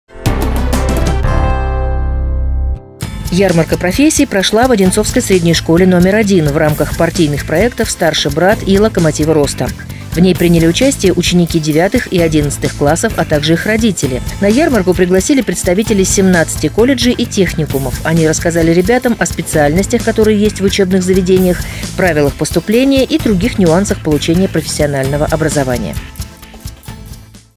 Правильный выбор
Ярмарка профессий прошла в одинцовской средней школе № 1 в рамках партийных проектов «Старший брат» и «Локомотивы роста». В ней приняли участие ученики 9 и 11 классов, а также их родители.